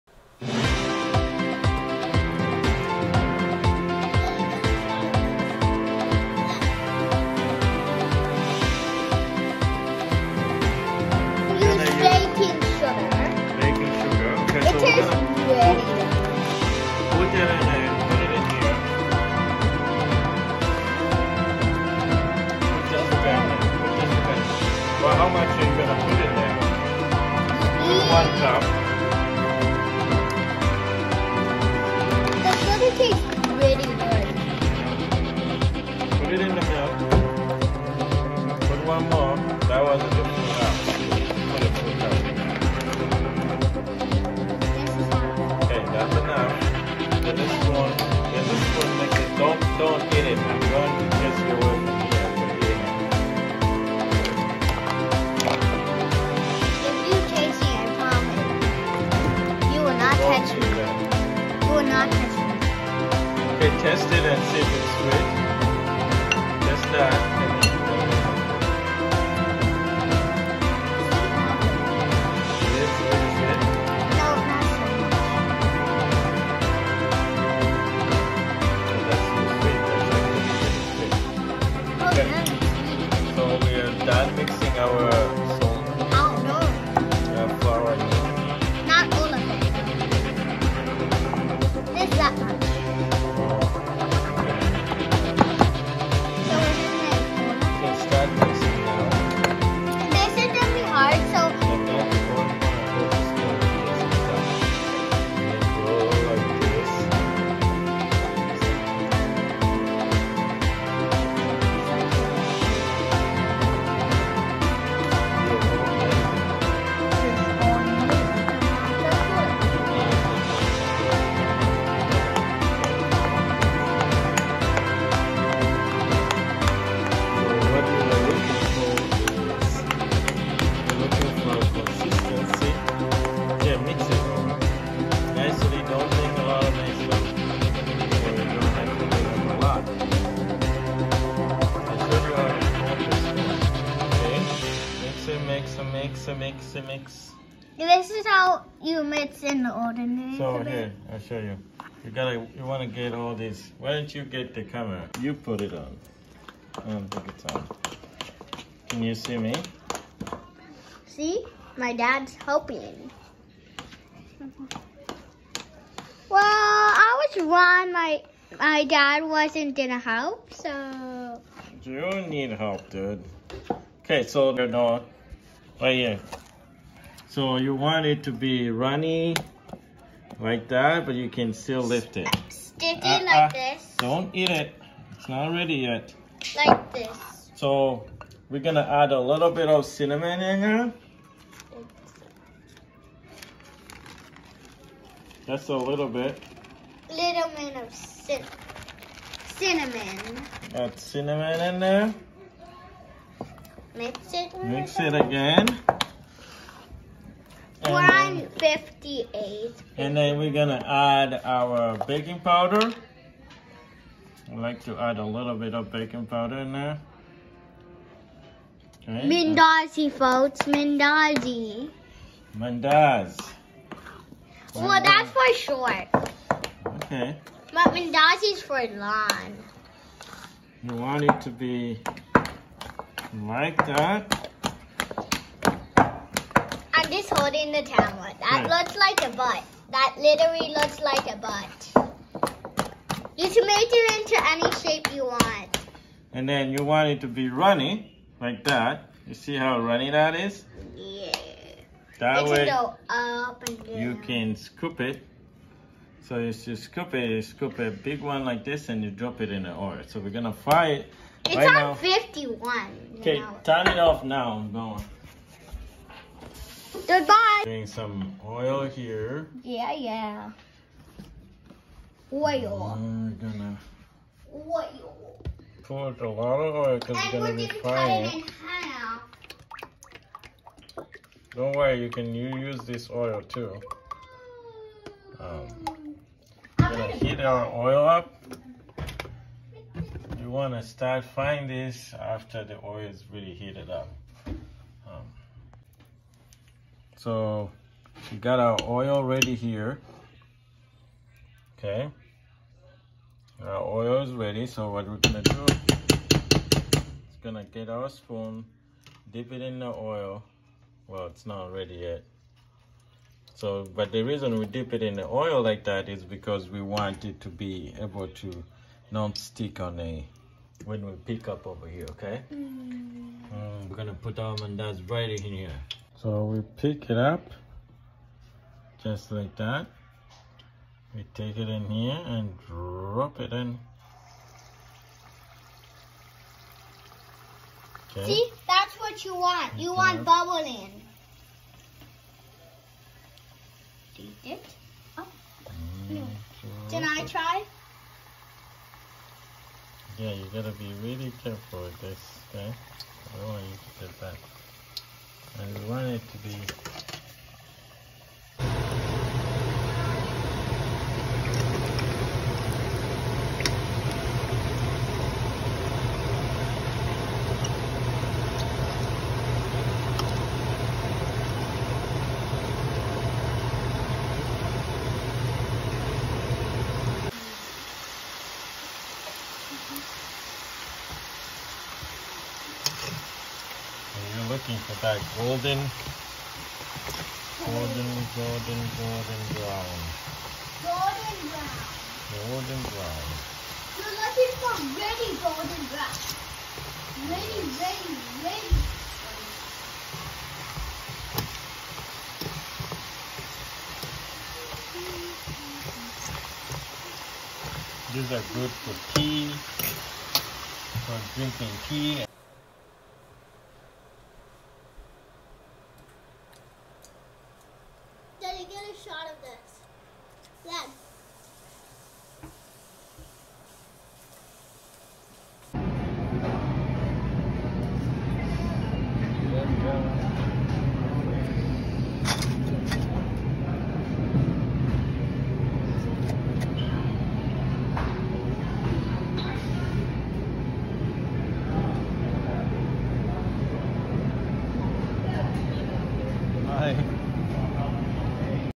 How to make Ugandan donuts
Making Ugandan donuts:

Ingredients!
Baking powder
Water or milk
Baking sugar
Flour
Cooking oil